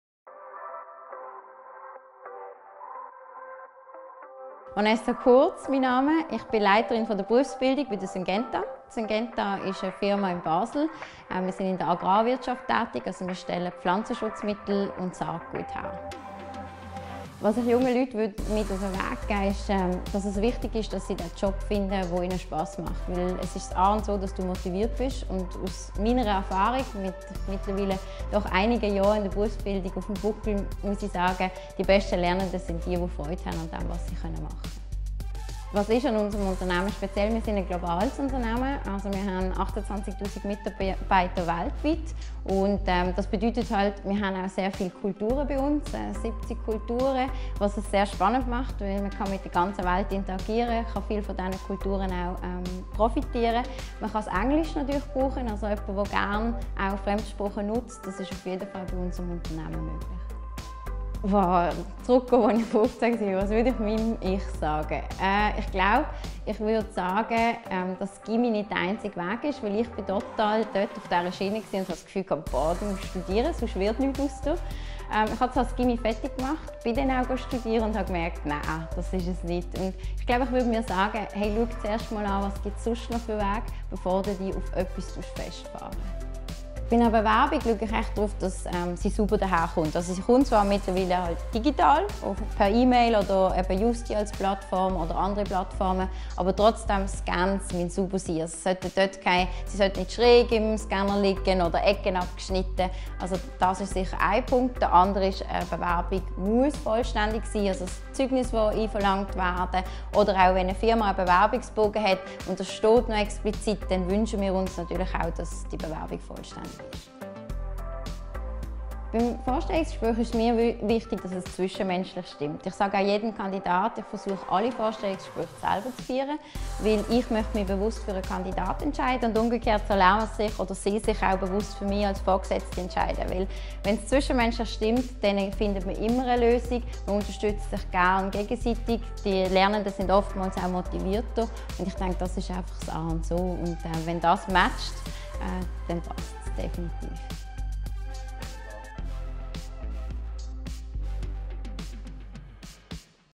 Insidertipps von Vanessa Kurtz - Syngenta Crop Protection AG
Vanessa Kurtz beantwortet diverse Fragen zur perfekten Bewerbung bei der Syngenta Crop Protection AG. Schau dir Ihre Insidertipps an und erstelle dein perfektes Dossier inklusive Lebenslauf.

Alle Informationen zur Firma Syngenta Crop Protection AG sind im Unternehmensprofil aufgelistet.